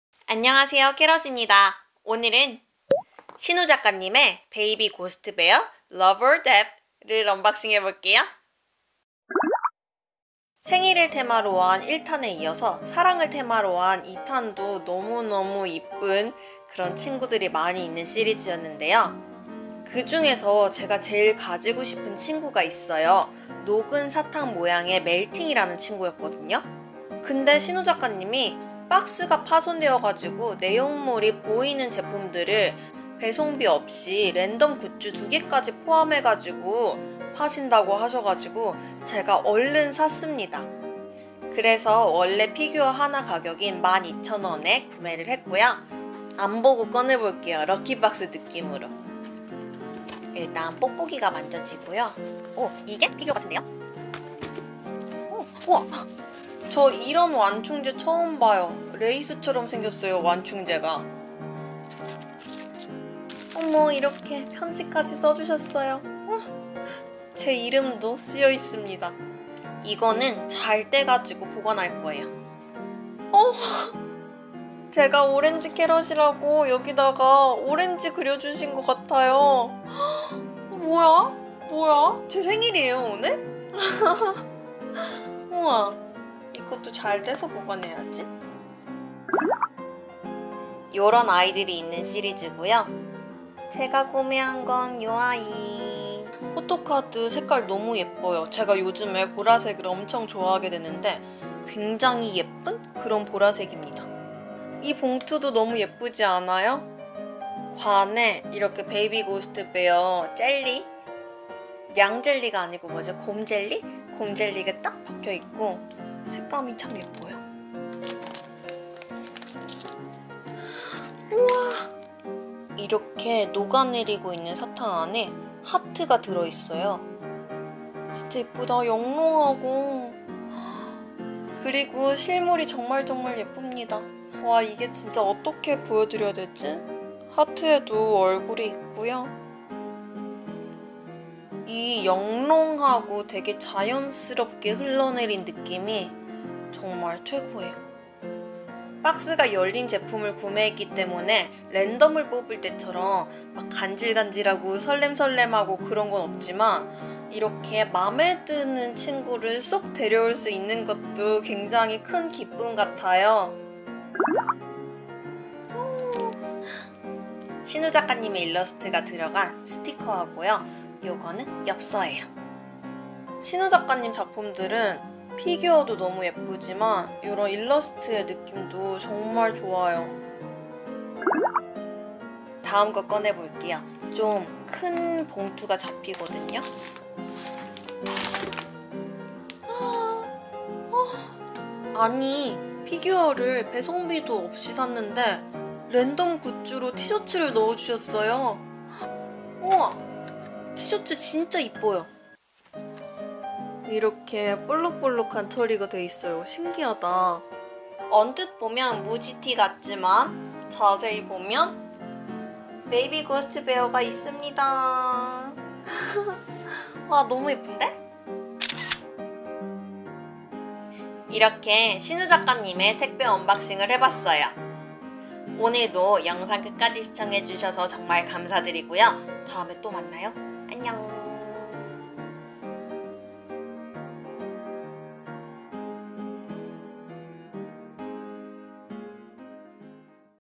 신우 작가님이 랜덤 박스를?????😍 베이비 고스트 베어 Love or Death🐾
안녕하세요! 키덜트 유튜버 오렌지캐럿🍊🥕입니다!

오늘은 신우 작가님의 베이비 고스트 베어 러브 올 데쓰! 피규어 리뷰입니다😍

피규어 말고도 대박 랜덤 아이템이 들어있으니 끝까지 재밌게봐주세요❤❤❤

인스타그램에도 놀러오세요 @orange_carrot_juice